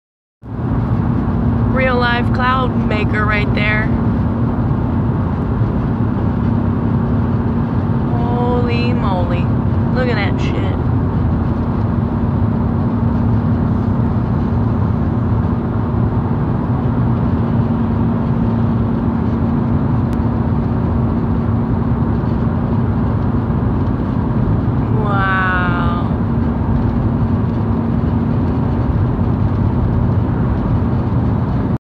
Cloud's or no?
My GAWD we've ALL BEEN TRICKED! Another reason besides gooberment forcing Globe down My throat why I believe in flat Earth. Land of Iowa